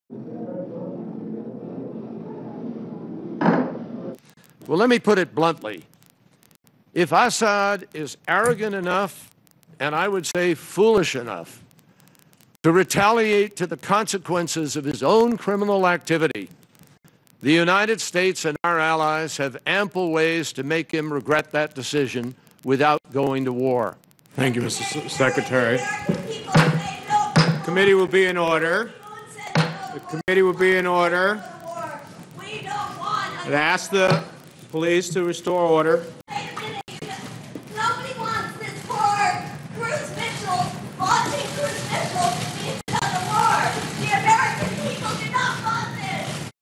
Protester heckles Senate as Kerry threatens Assad
W/S Start of United States Senate Foreign Relations Committee hearing

SOT: John Kerry - US Secretary of State (sound corrupted at source): "Let me put it bluntly: If Assad is arrogant enough, and I would say foolish enough, to retaliate to the consequences of his own criminal activity, the United States and our allies have ample ways to make him regret that decision without going to war."

W/S Protester disrupts US Senate Foreign Relations Committee hearing UPSOUND: protester interrupting hearing

W/S US Senator Robert Menendez looks on as a protester Senate Foreign Relations Committee hearing UPSOUND: protester interrupting hearing

SCRIPT

USA: Protester storms Senate as Kerry threatens Assad

The United States has "ample ways to make" Assad to regret his decision, if Syrian government forces retaliate against a US attack, US Secretary of State John Kerry said on Tuesday in Washington. Kerry was at a Senate Foreign Relations Committee hearing about how the US will respond to the alleged use of chemical weapons by the Syrian government last month.

Kerry stated: "If Assad is arrogant enough, and I would say foolish enough, to retaliate to the consequences of his own criminal activity, the United States and our allies have ample ways to make him regret that decision without going to war".

Soon after Kerry finished his speaking, a protester stormed into the chamber shouting "Nobody wants this war." The protester was soon escorted out by police. US President Barack Obama has said US ground forces will not be deployed against Syria.